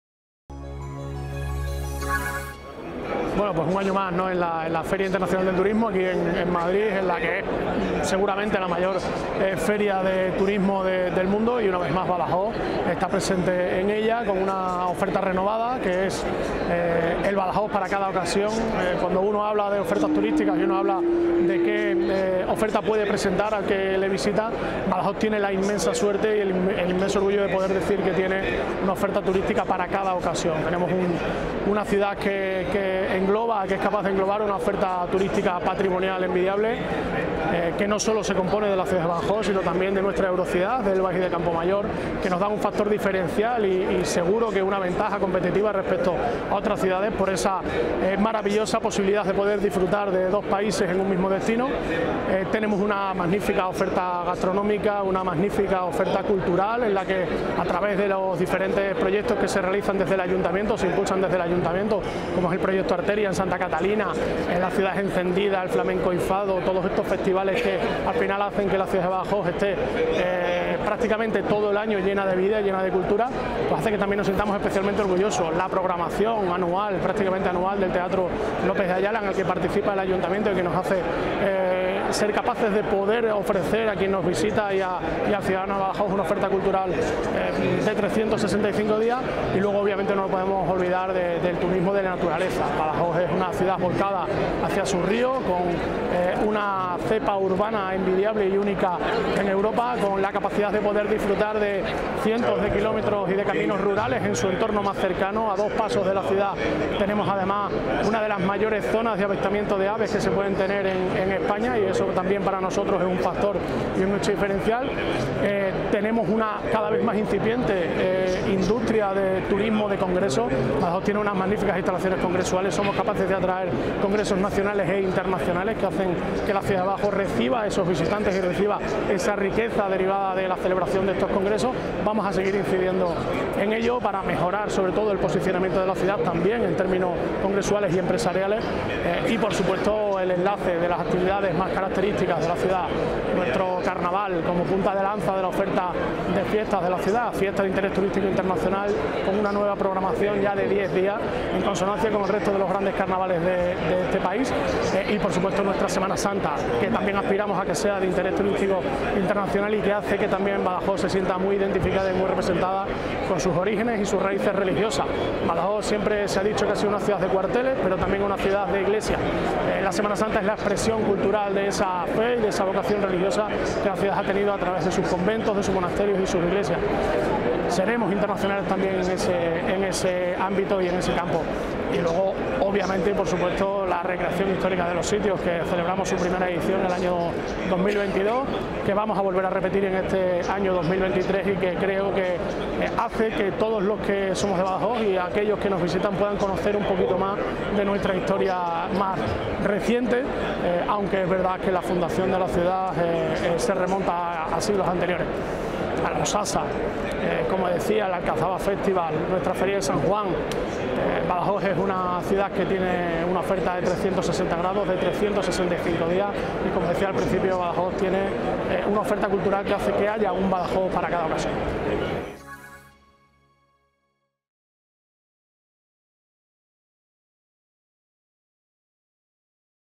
Rueda de prensa Alcalde - Presencia de Badajoz en FITUR
El alcalde de Badajoz, Ignacio Gragera, comenta la presencia de la ciudad en FITUR 2023.

Un contenido producido por RECarmy.com Productora Audiovisual Badajoz Extremadura ) para Badajoz Online Tv la Televisión Municipal de Badajoz.